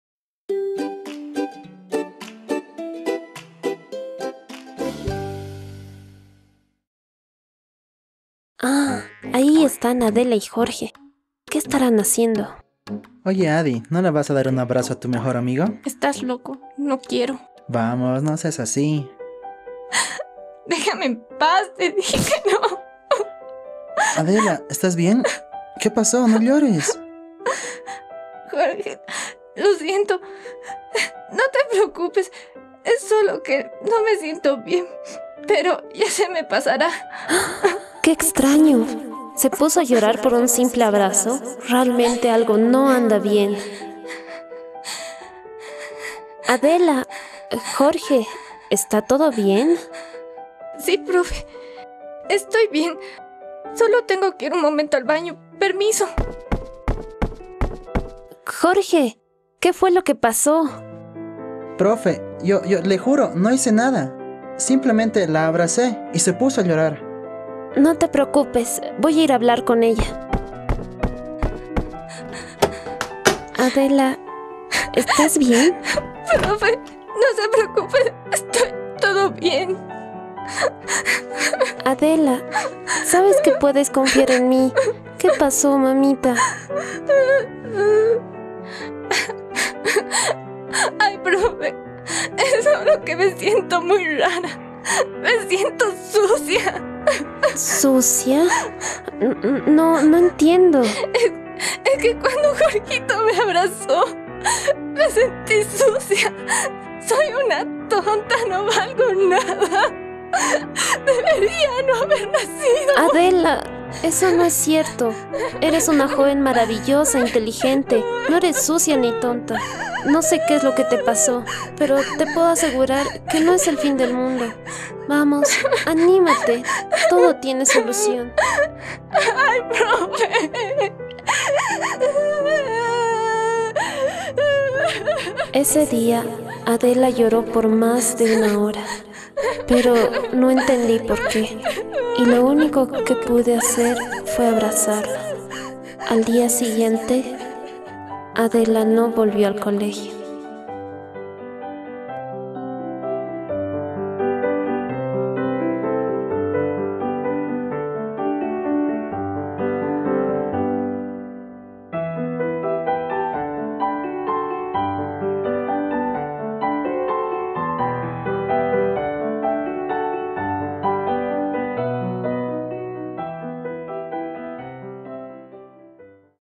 El Diario de Adela (Capítulo 2 - Parte 2)
Acá va la segunda parte del Capítulo 2. Margarita encuentra a Jorge y Adela en el patio del Colegio... pero algo pasa con Adela y...